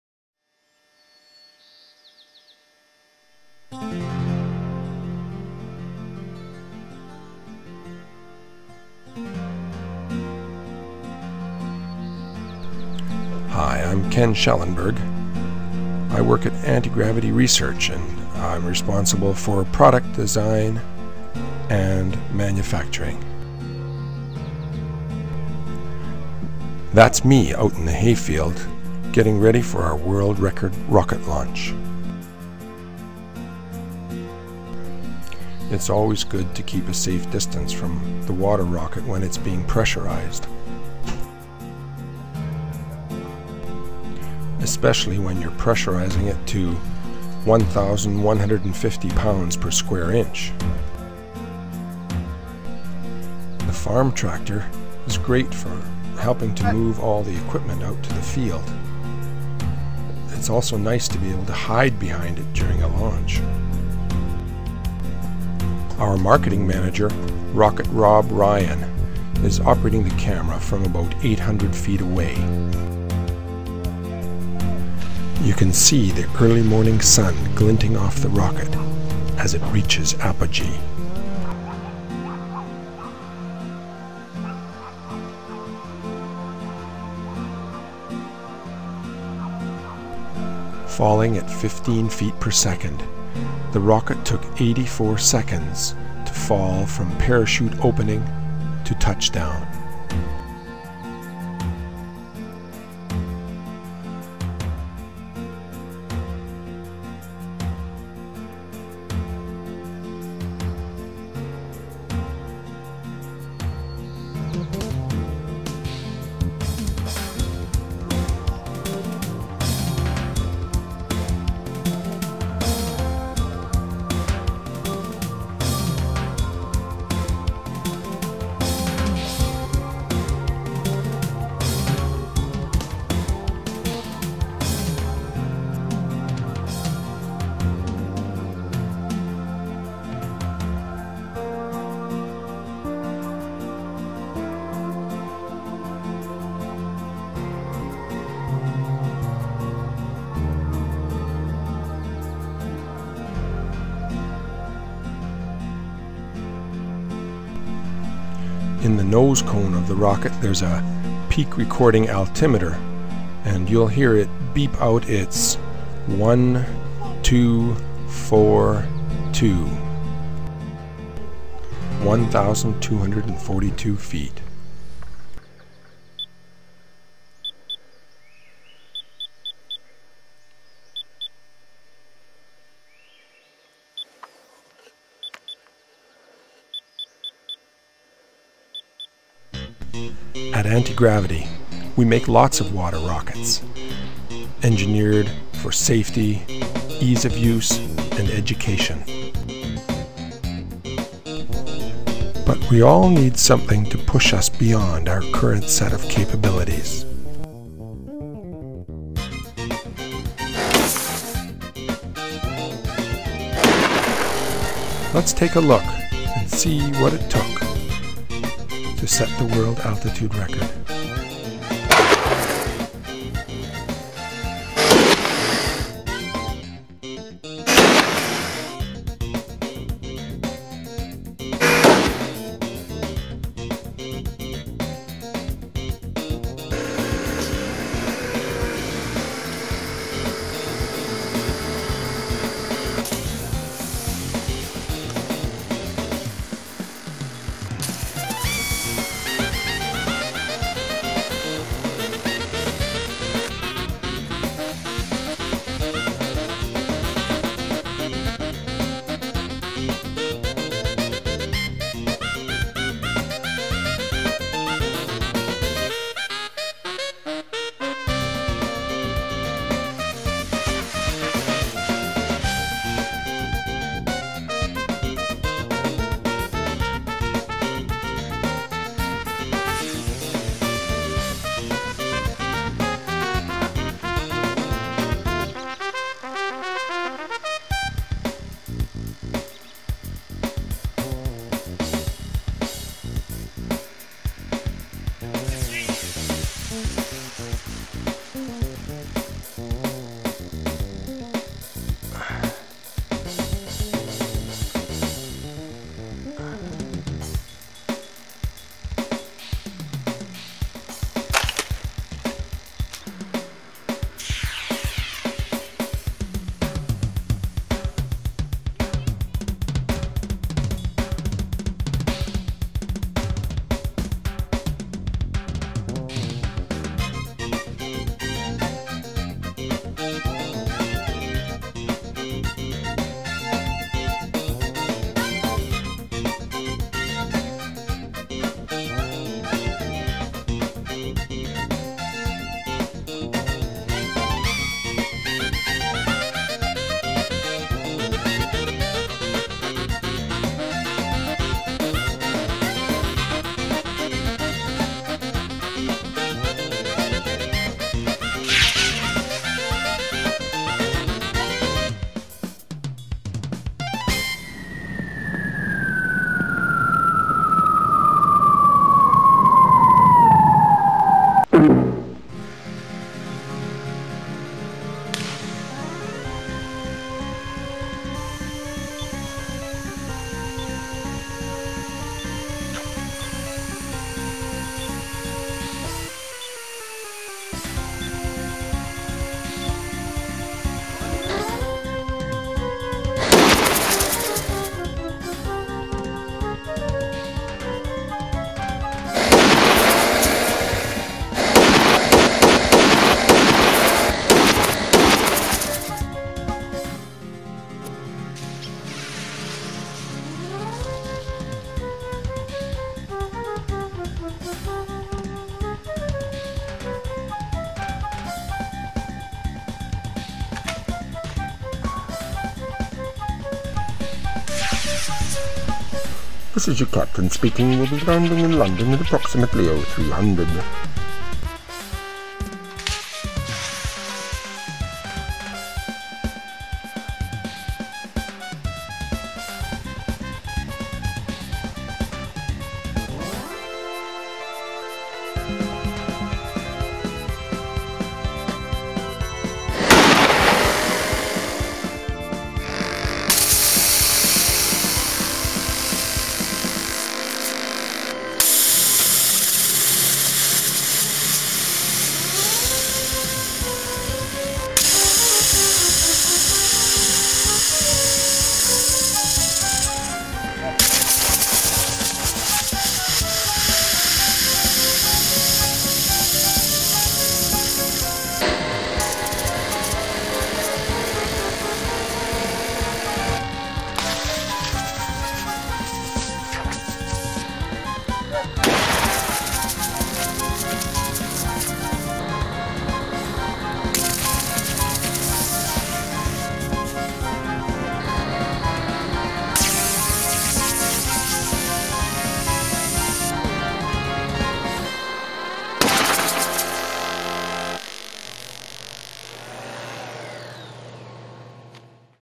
World Record Pop Bottle Rocket Part 1
Part 1 of the World Altitude Record for pop bottle rockets.  Plenty of crashes, smashes, explosions and failures, followed by success!  Make sure to see all three parts of this video!